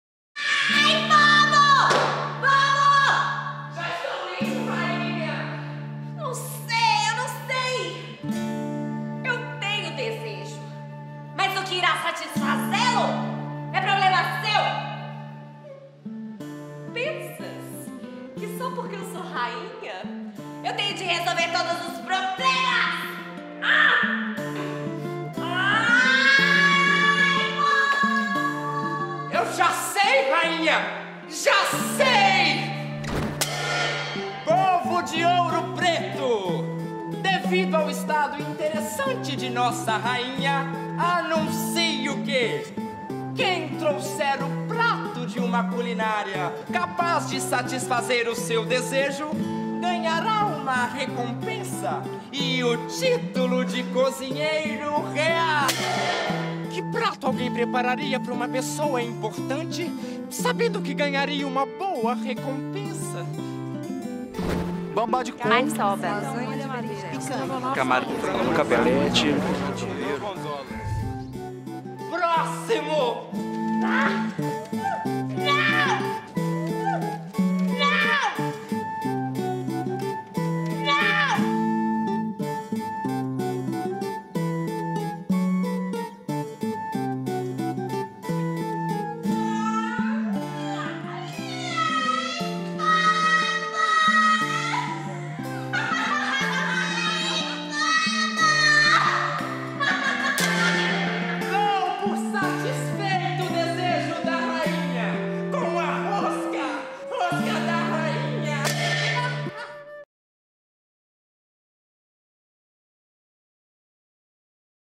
Geração Futura - A Rosca da Rainha
Produção da TV UFOP em parceria com o Canal Futura, produzido pela estudante Josiane Reis, durante o curso de audiovisual Geração Futura .